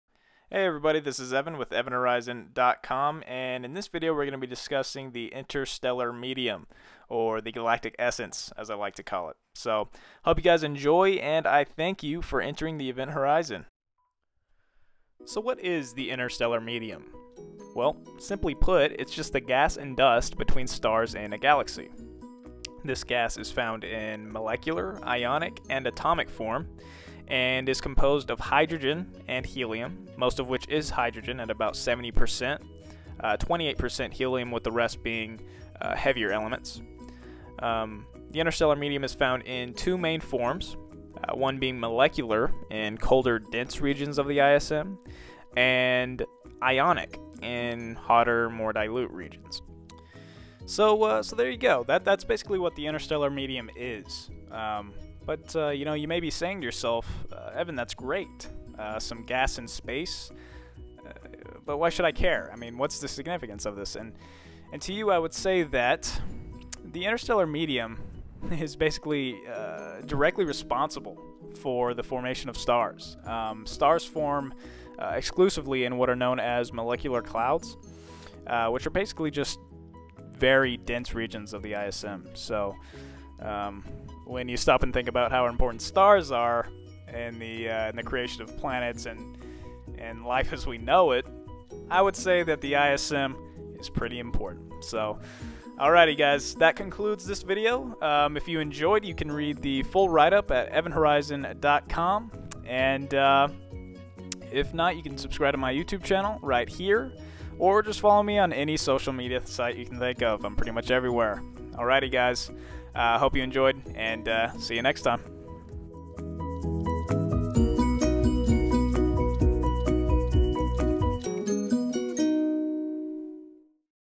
What is the Interstellar Medium?
In this video, we explore what lies between the stars, the interstellar medium. What is it?  Why is it significant? Enter the Evant Horizon and find out!  -photos by NASA